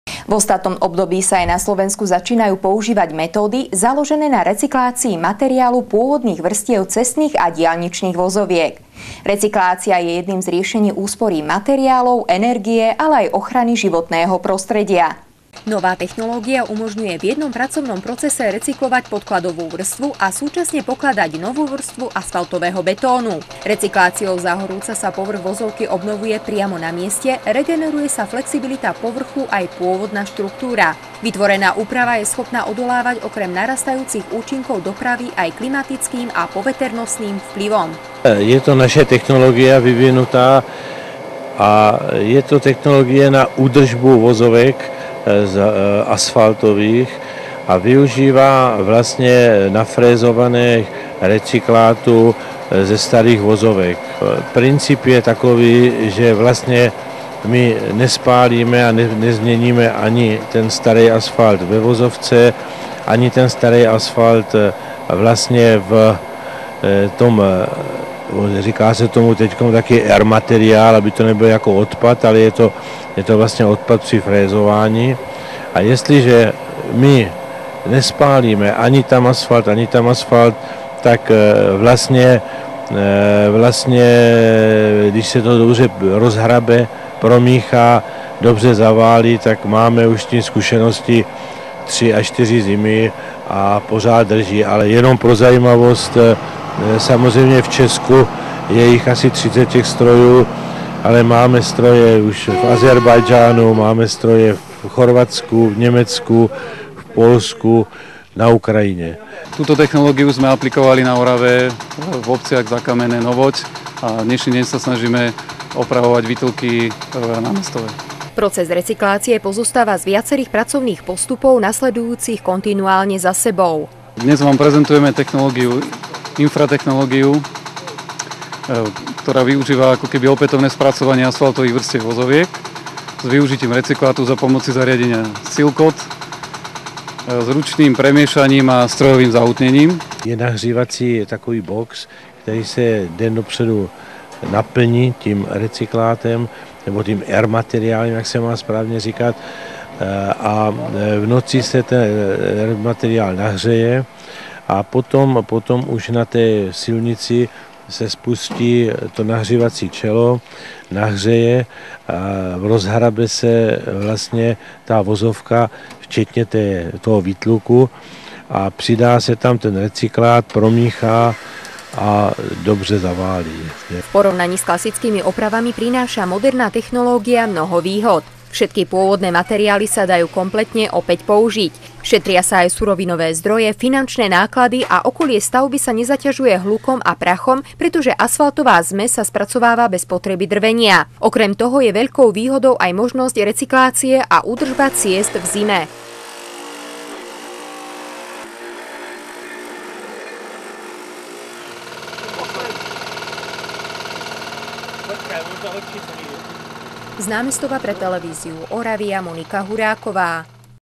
Nová moderná technológia na údržbu ciest
V ostatnom období sa aj na Slovensku začínajú používať metódy založené na recyklácii materiálu pôvodných vrstiev cestných a diaľničných vozoviek. Recyklácia je jedným z riešení úspory materiálov, energie, ale aj ochrany životného prostredia
Nová  technológia umožňuje v jednom pracovnom procese recyklovať podkladovú  vrstvu a súčasne pokladať novú  vrstvu asfaltového betónu. Recykláciou zahorúca sa povrch vozovky   obnovuje  priamo na mieste, regeneruje sa flexibilita povrchu aj pôvodná štruktúra. Vytvorená úprava je schopná odolávať okrem narastajúcich účinkov dopravy aj klimatickým a poveternostným vplyvom.